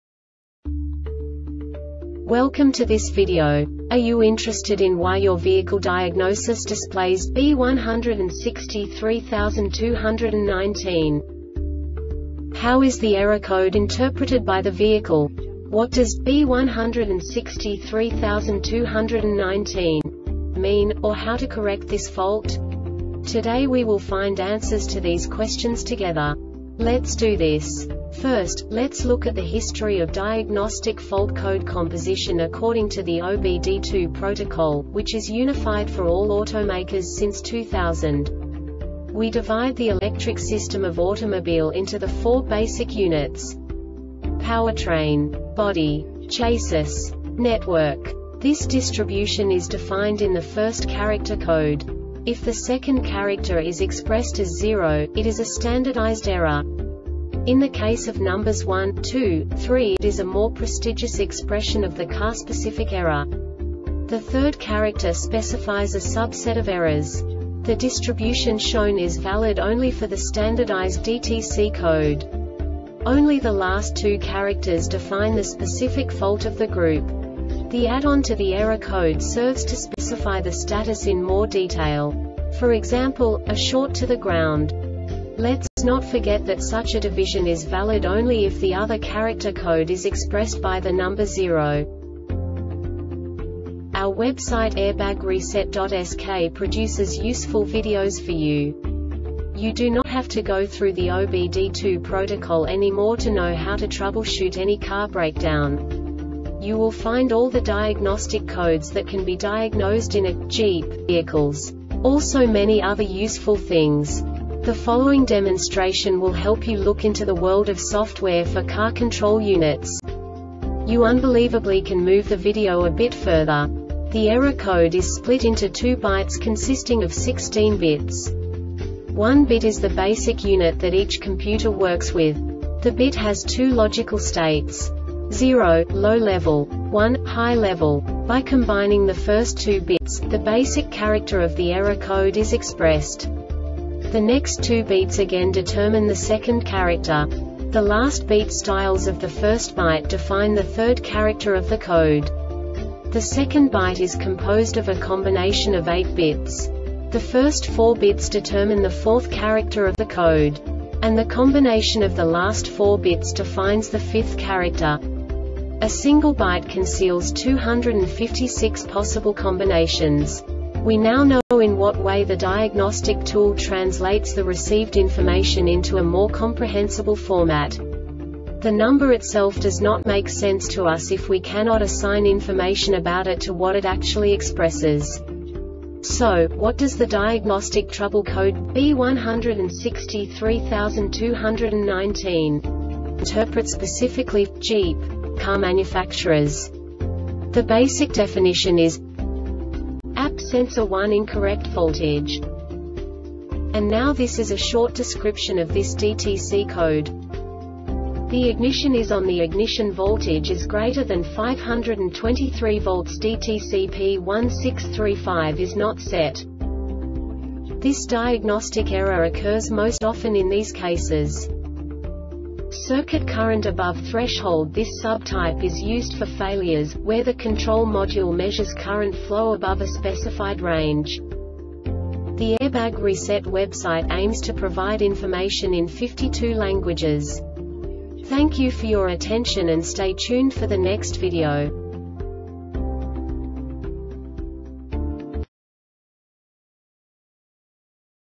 DTC Jeep B1632-19 Short Explanation
Contents:
0:21 Basic DTC analysis according to OBD2 protocol standard.
1:48 Insight into programming
2:58 A diagnostically understandable description of the Diagnostic Trouble Code
3:05 Basic error definition

Make:
Jeep
Code:
B1632 19
Definition:
APP Sensor 1 Incorrect Voltage
Description:
The ignition is ON.The ignition voltage is greater than 5.23 volts.DTC P1635 is not set.